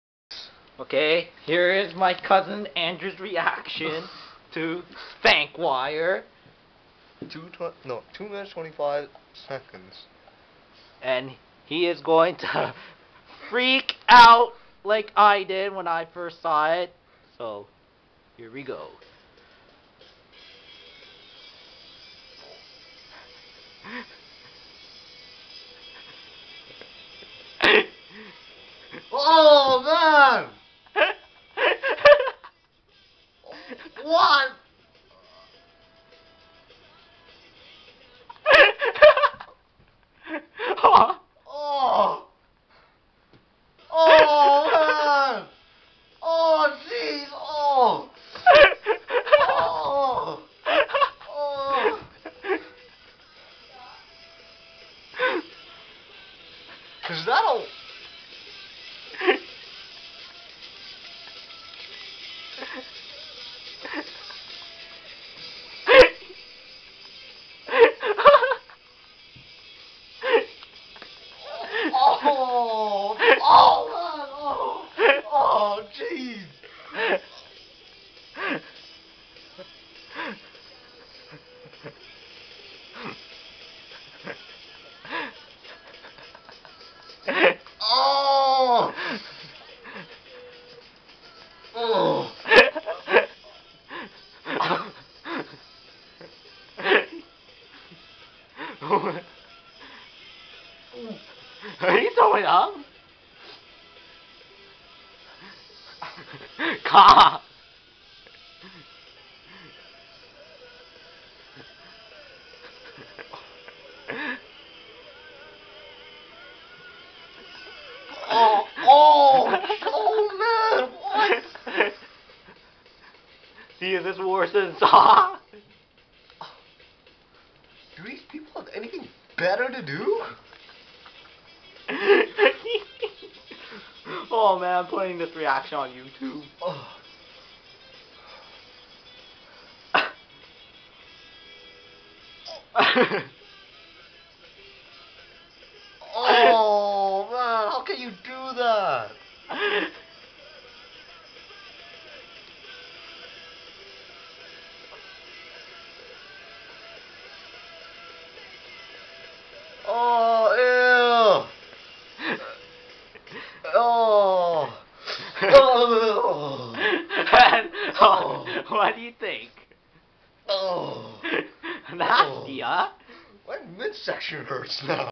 spankwire reaction
i recorded my cousin andrew watching the spankwire video and his reaction was lol